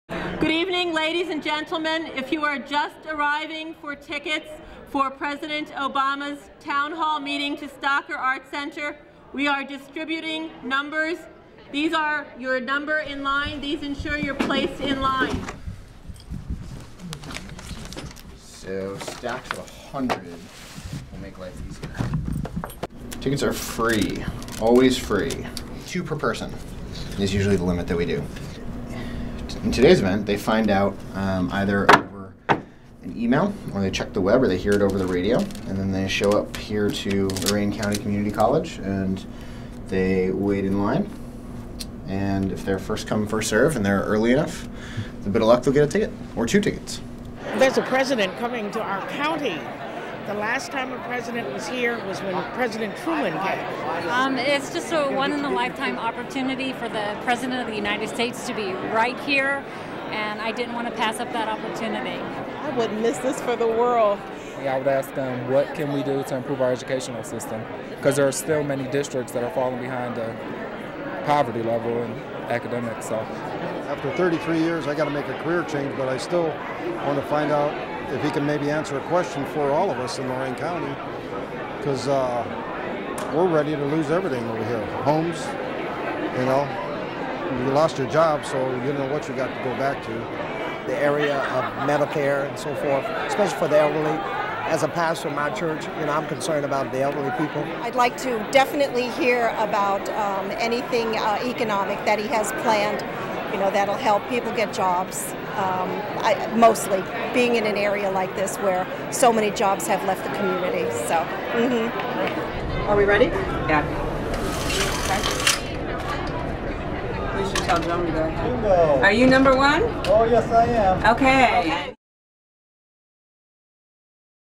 Tickets Released for Lorain County Town Hall
Tickets have been released in Lorain County, OH for this Friday's Town Hall with President Obama. This will be the first time a sitting president has visited the community, hard hit by the economic downturn, since Truman.
Public Domain